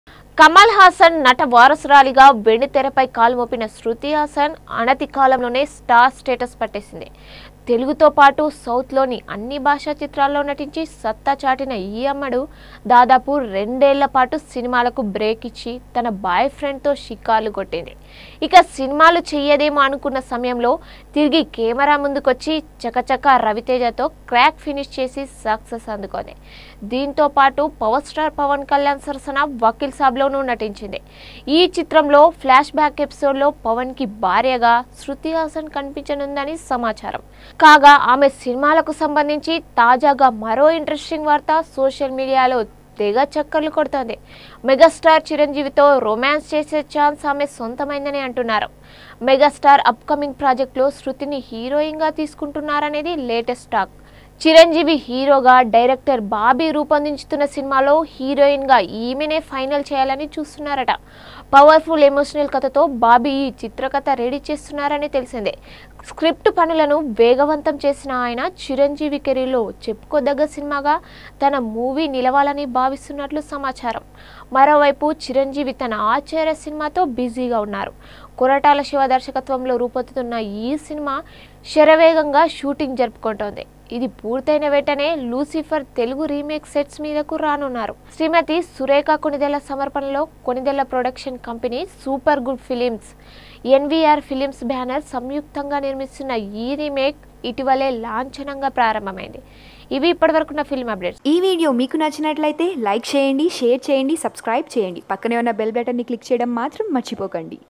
చిరంజీవి తో శృతిహాసన్ | Shruti Hassan Will Acts In Megastar Chiranjeevi Next Movie
Hey guys!